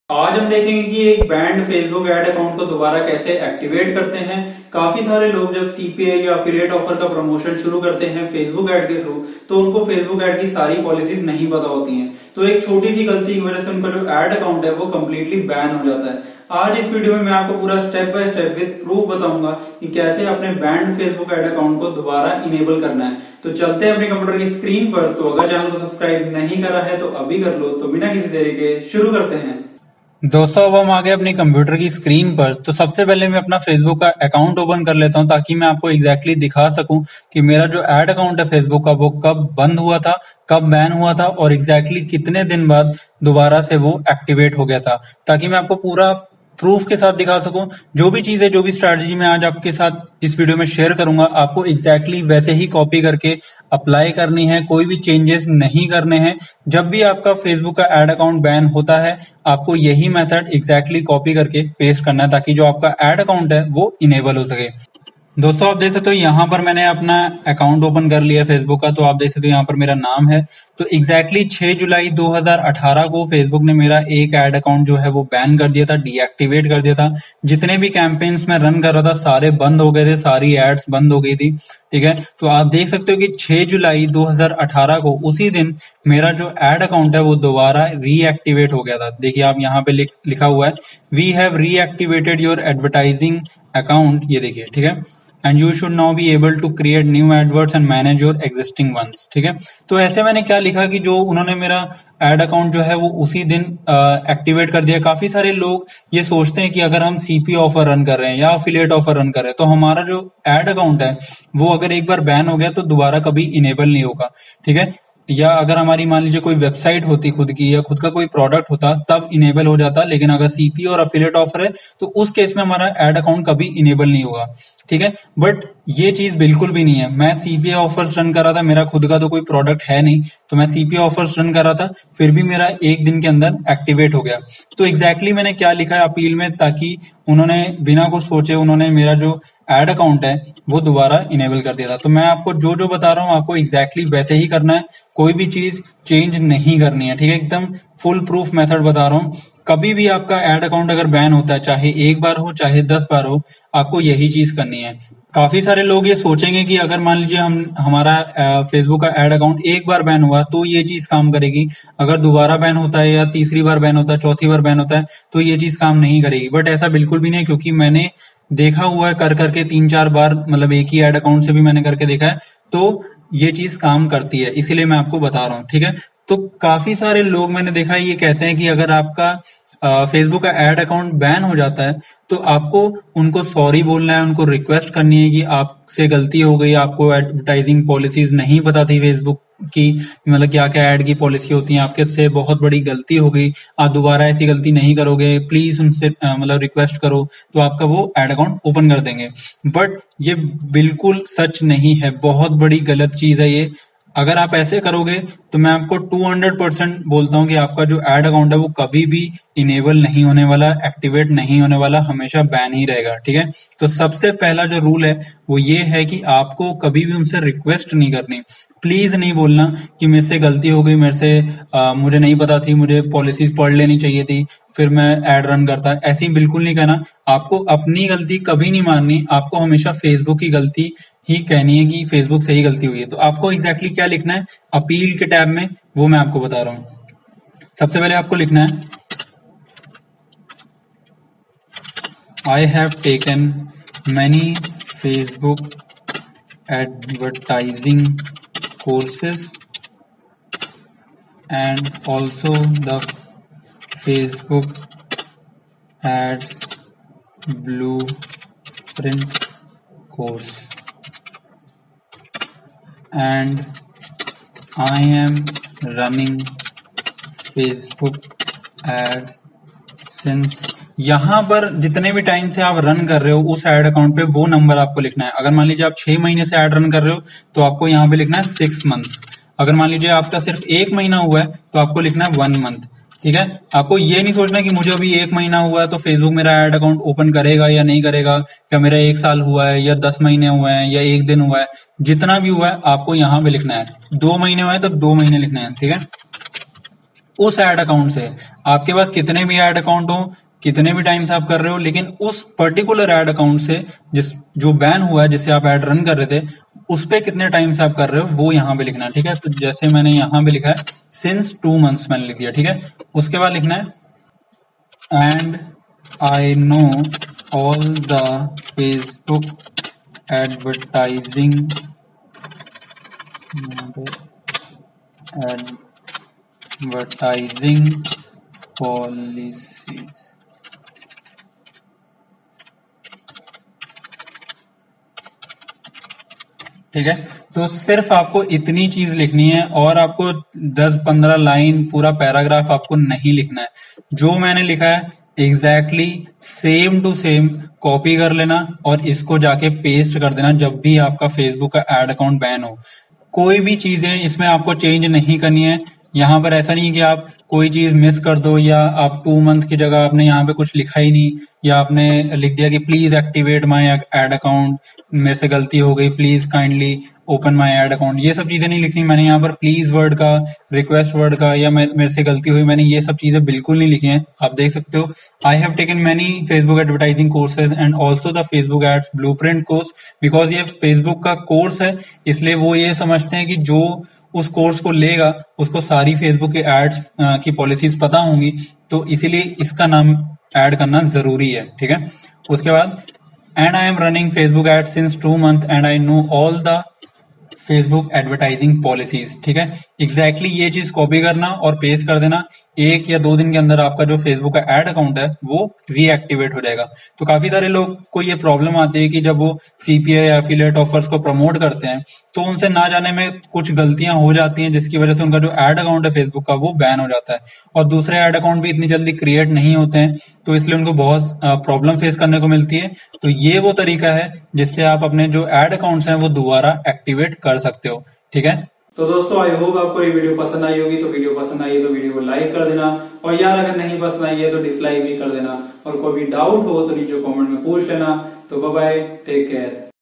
How to Recover a Banned Facebook Ad Account [ With Proof ] | Facebook Ad Account Disabled
Hello, In this video I am going to tell you How to Recover a Banned Facebook Ad Account [ With Proof ]. A lot of people when start promoting CPA offers through facebook ads due to some policy violation their Facebook Ad Account Disabled. So today in this video I am going to show you How to Recover a Banned Facebook Ad Account [ With Proof ]. So watch this video till the end.